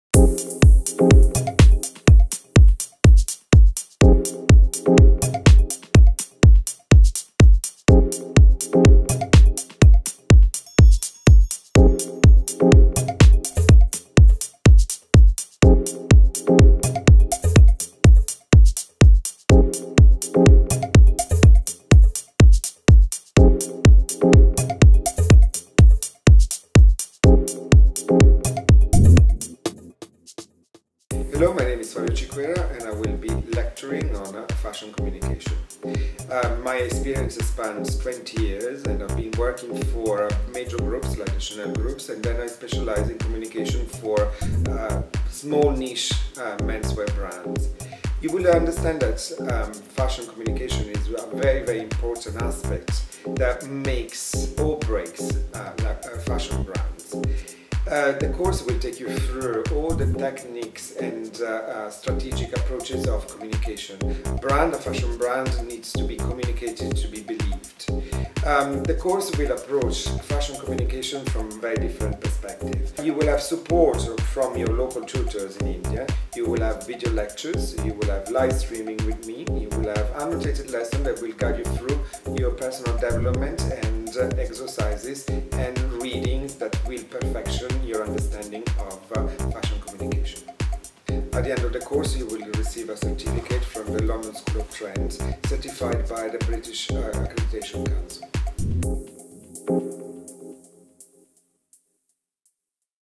Online Strategic Fashion Communication Short Programme
Mr Fabio Ciquera explaining the short online programme